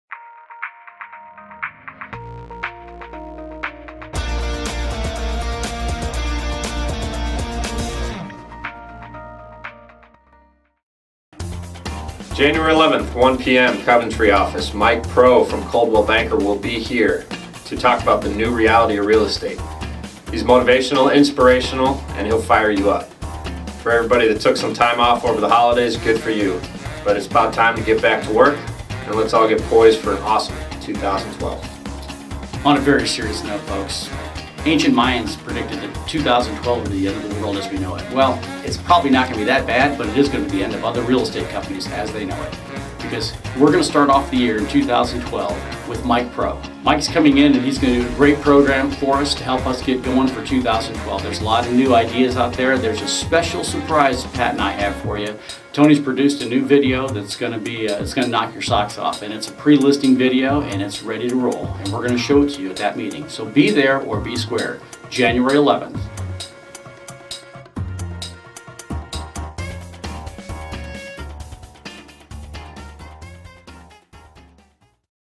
January 11 All Company Meeting w/ Mike Pro
Coldwell Banker Roth Wehrly Graber welcomes real estate expert Mike Pro to offer new tips and tools for getting your business to the next level in 2012.